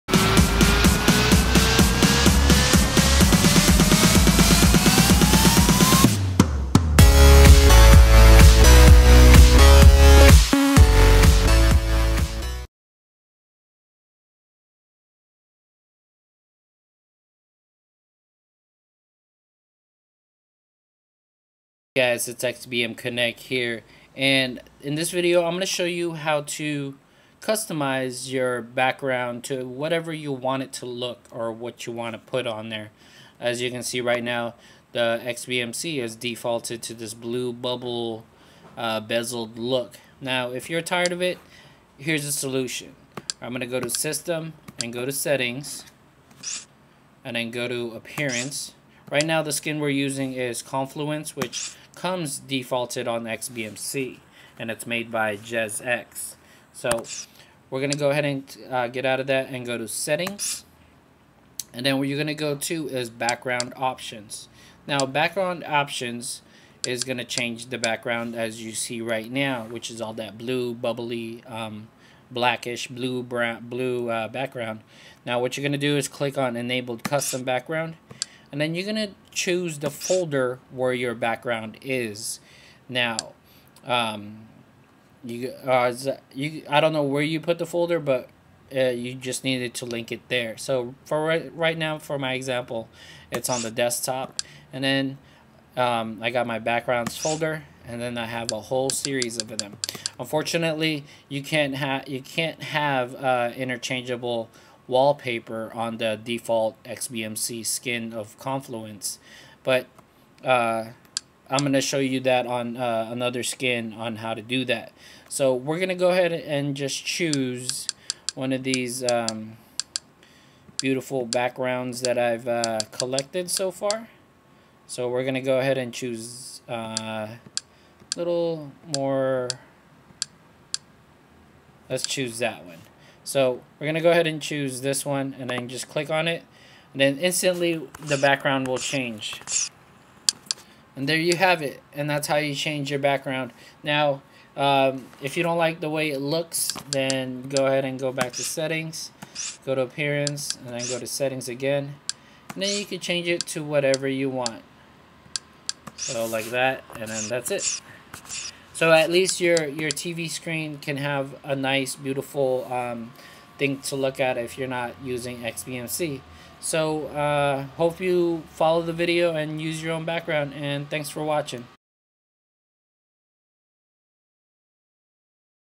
How to change your background on XBMC:KODI
This is a video tutorial on how to change your xbmc / kodi background into your own custom background.